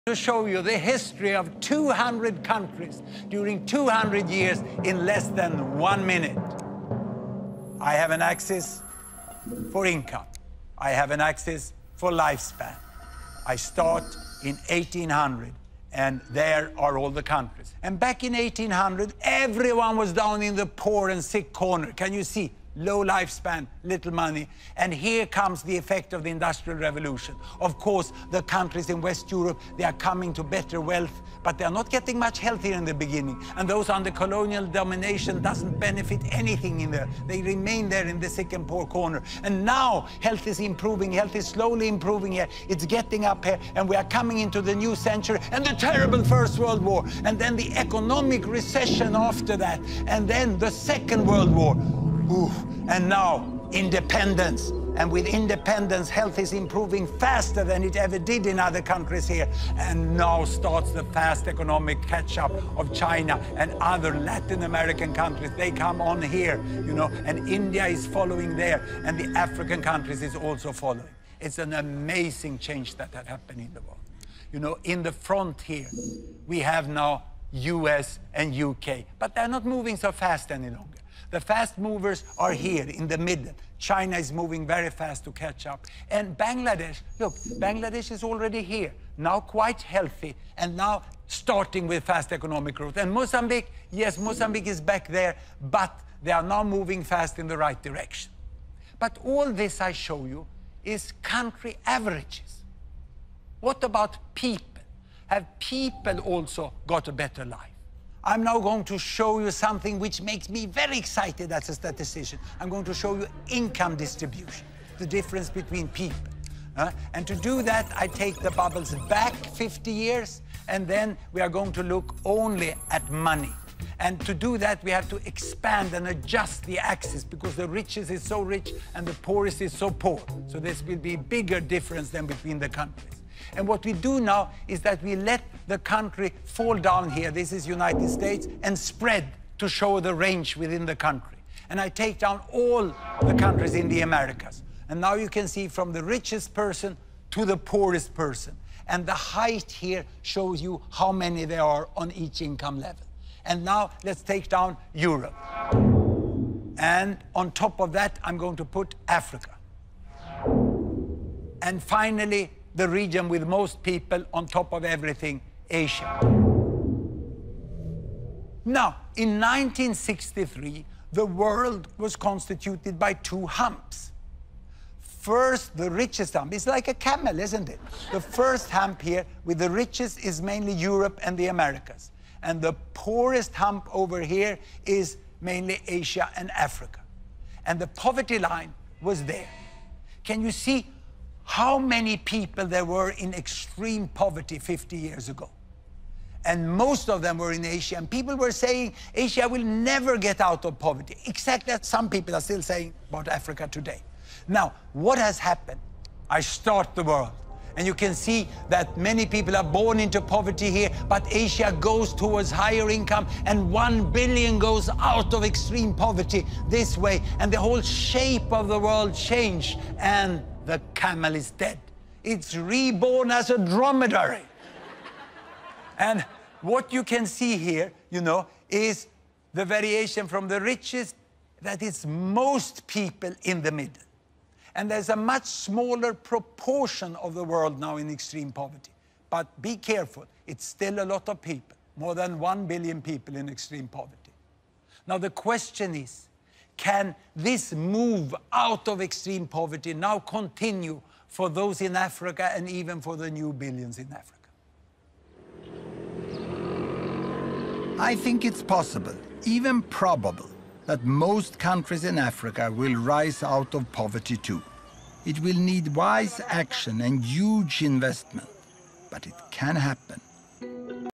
Hans Rosling 200 länder på 200 år
Hans Rosling beskriver industrialismens betydelse för folkhälsan och sedan hur den gamla indelningen i I-länder och U-länder inte riktigt håller. "Kamelen är död..."